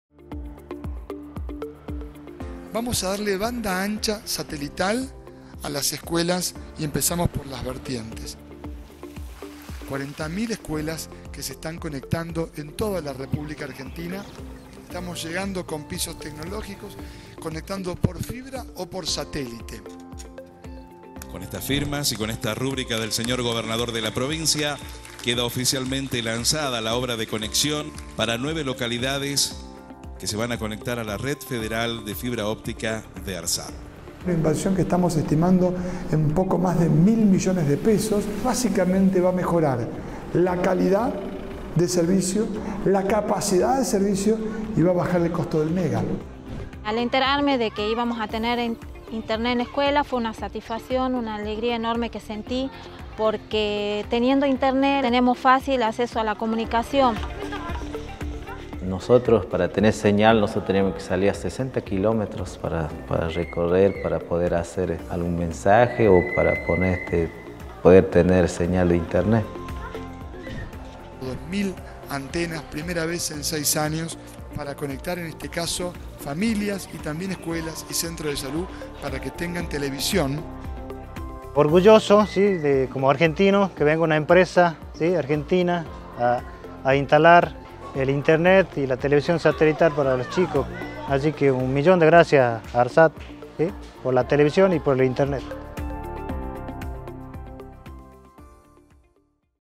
Conectividad en Salta
Estuvimos presentes en la provincia de Salta para relevar y mejorar la conectividad de la zona. Llegamos hasta Las Vertientes, un pueblo de 120 familias, en la escuela Nro. 4199, instalamos antenas que brindan internet satelital y televisión digital. Fue la primera vez que la institución tiene conectividad y está comunicada con el resto del país. Llegar a aquellos lugares donde no es fácil acceder, es dar un paso firme para achicar la brecha digital.